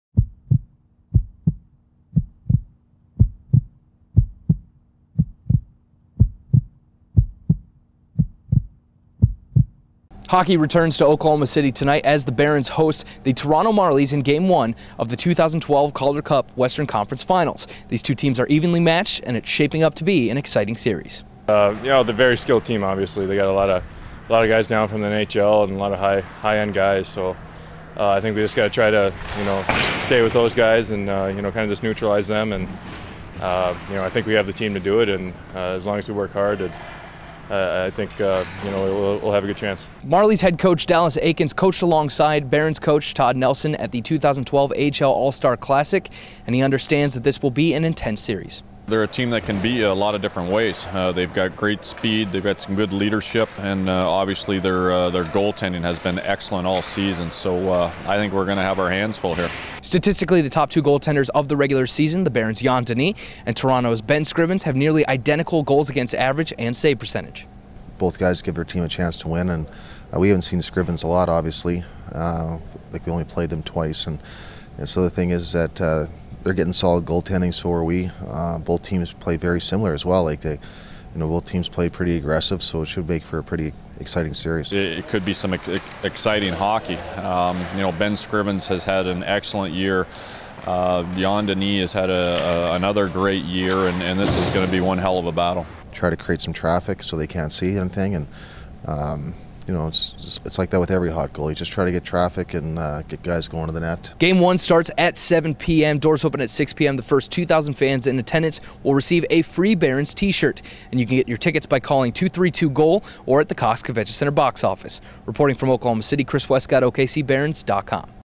Barons Beat: OKC Opens Western Conference Finals
OKCBarons.com's Chris Wescott reports on the Barons as they open up the Western Conference Finals tonight.